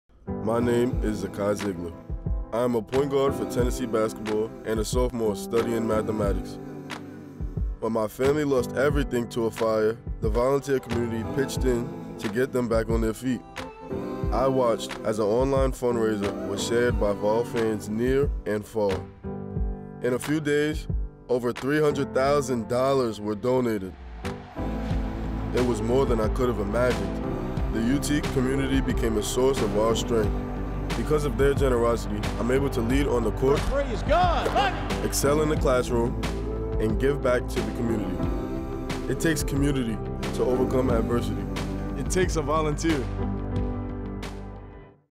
It Takes A Volunteer—Zakai Zeigler
Last February, mathematics major and point guard for Tennessee basketball Zakai Zeigler received heartbreaking news—his family’s home in Queens, New York, had been destroyed in a fire.  Vols near and far pitched in with donations and support, and their generosity helped the family get back on their feet. Zeigler credits the Volunteer community with fueling his work on the court and in the classroom as well as his service in programs like Hoops for Hope, which benefits the Down Syndrome Awareness Group of East Tennessee.

utknoxville is on your favorite social media channels